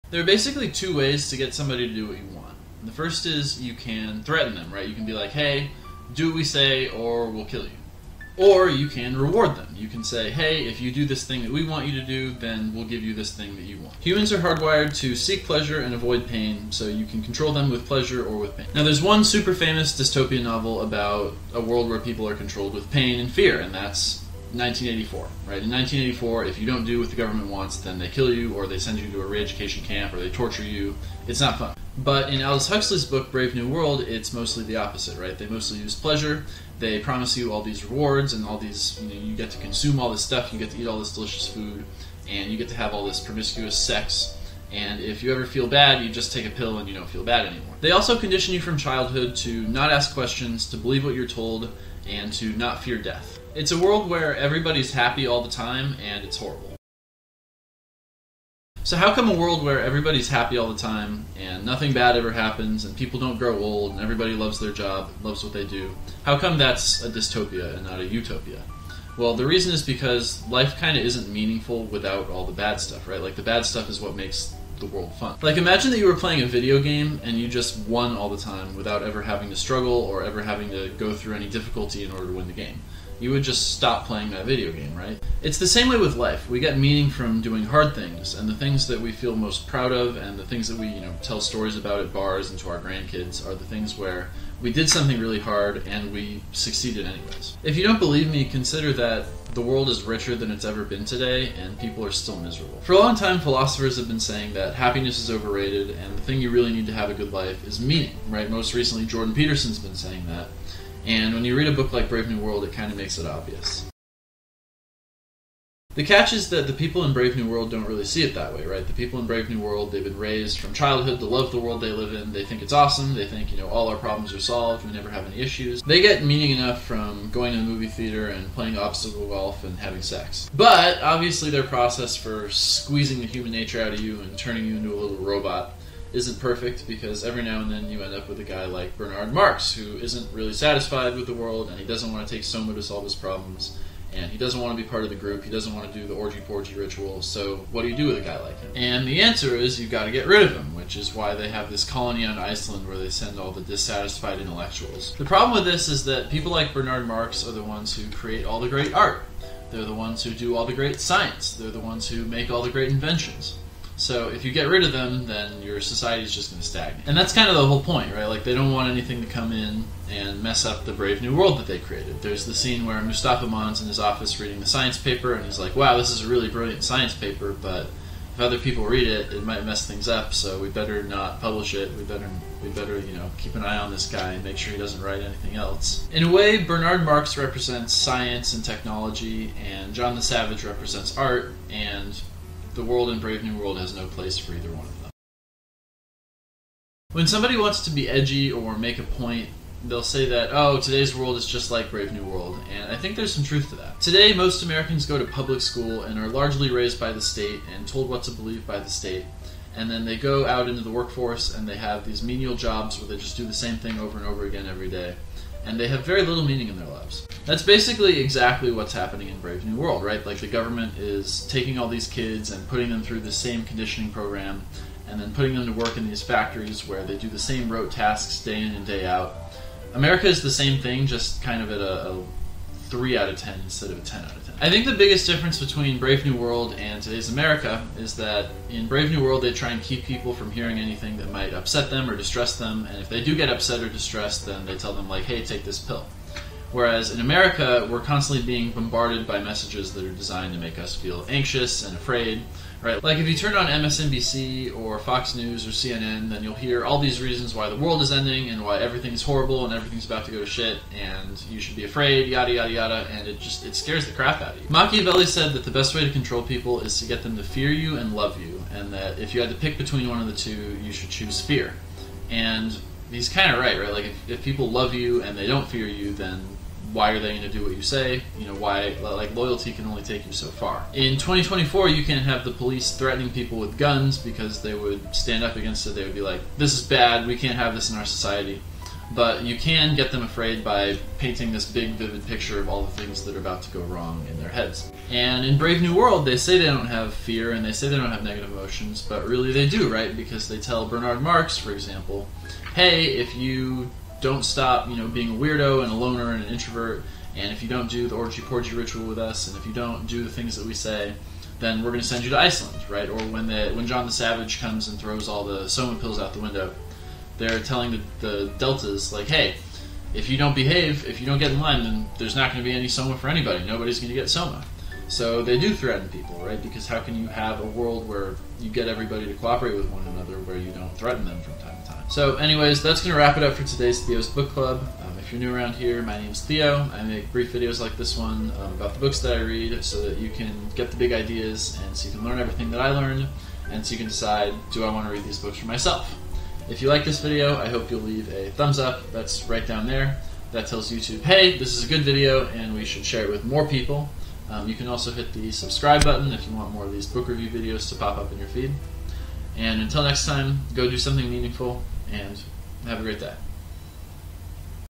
Book Summary — Brave New World by Aldous Huxley
If the world was perfect and nobody had any problems then it would be terrible. There would be no art, no science, and life would quickly lose all meaning.

That's Aldous Huxley's point in his book "Brave New World".